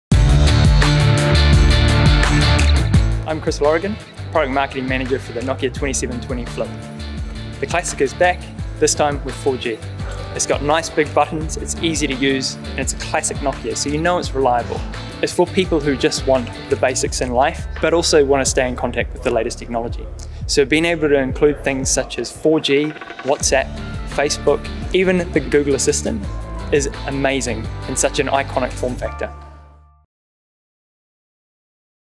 Let's talk all things Nokia 2720​ Flip
The classic flip is back. Hear all about its modern revival with Chris, our Product Marketing Manager.

▶️nokiaphon.es/Nokia2720Flip